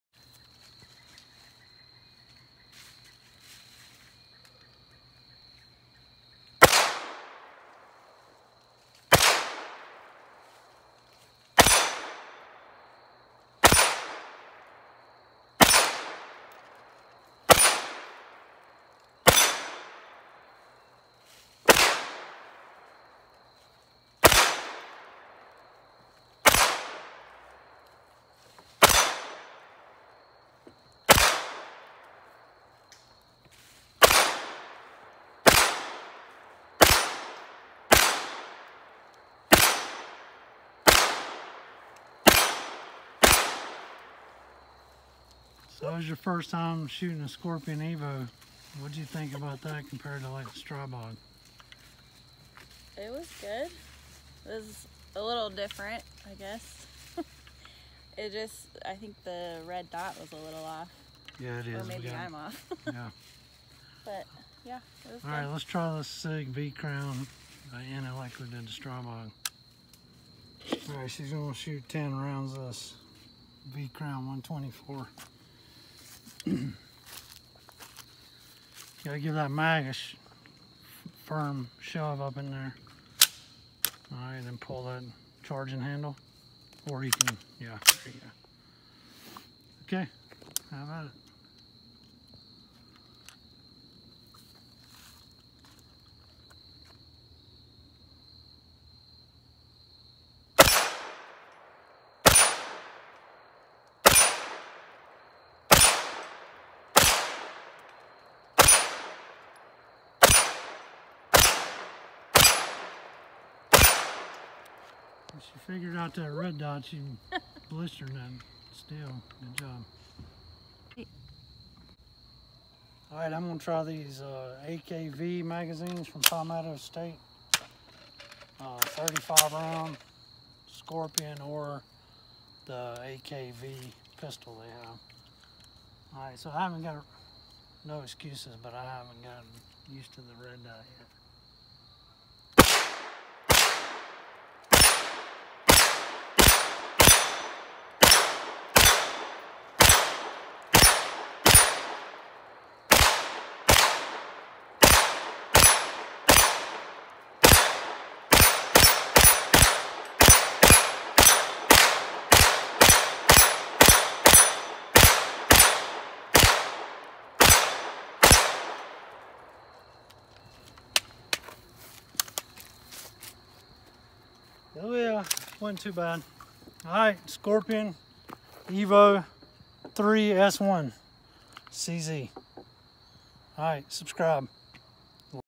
CZ Scorpion EVO 3 PCC Ammo Test
Running a few ammo options in the Scorpion EVO PCC including the sig v crown self defense hollow point 124 grain ammo. Also testing the PSA AKV magazine.